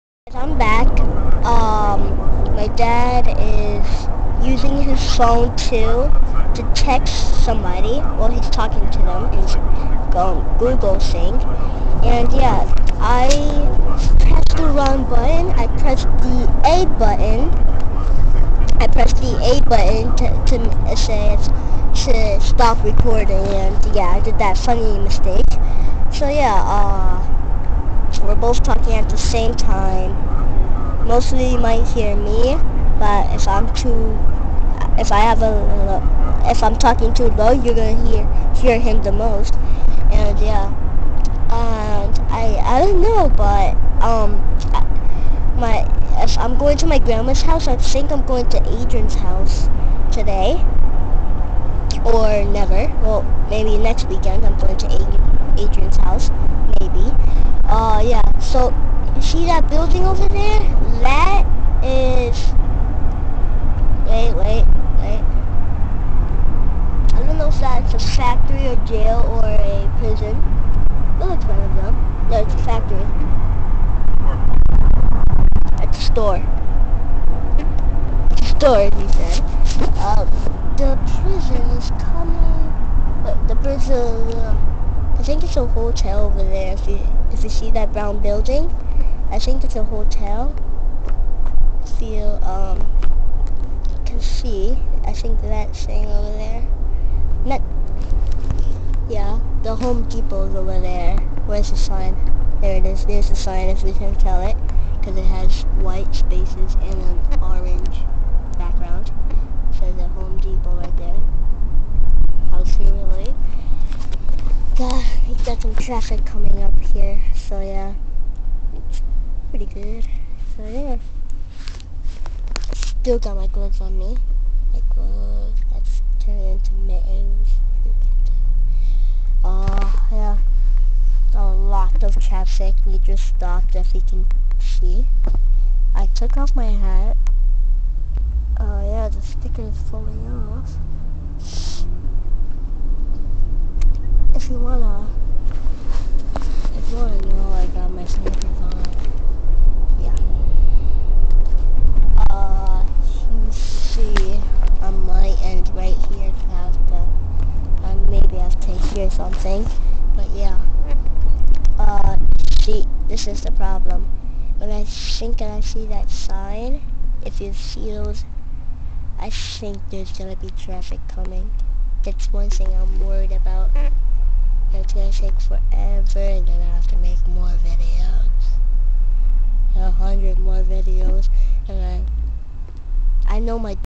Jordan's Video Blog - 8 Years Old - Video 5 - 10.10.2015.AVI
Jordan records on his Nintendo 3DS about his family, apartment, and his move.